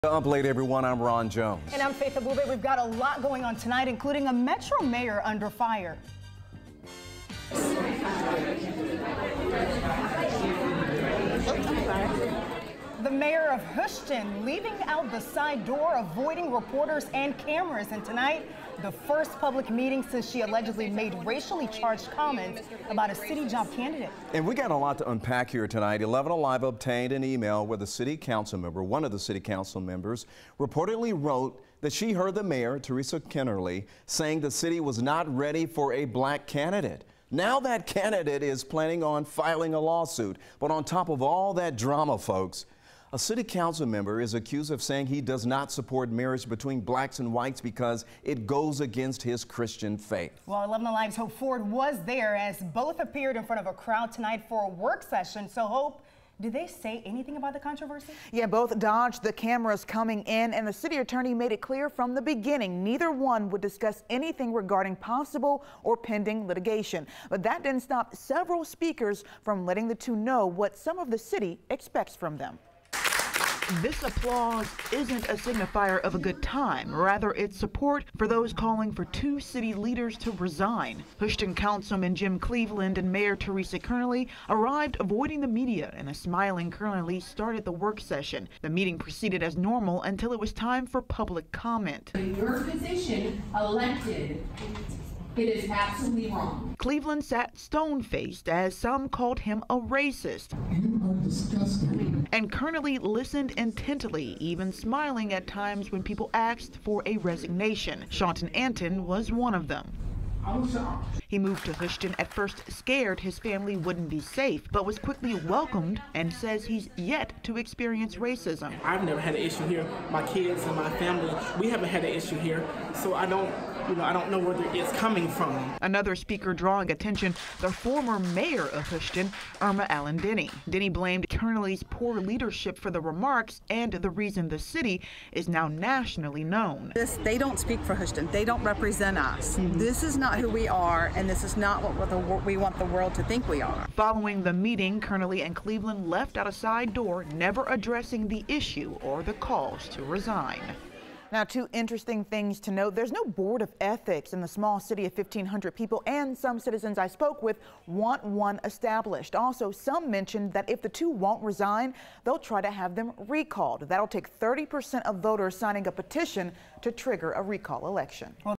'You're disgusting,' Citizens call for resignation of city leaders over alleged racist remarks
A mayor and councilman face calls of racism and resignation after allegedly making racists remarks about a job candidate and interracial marriage.v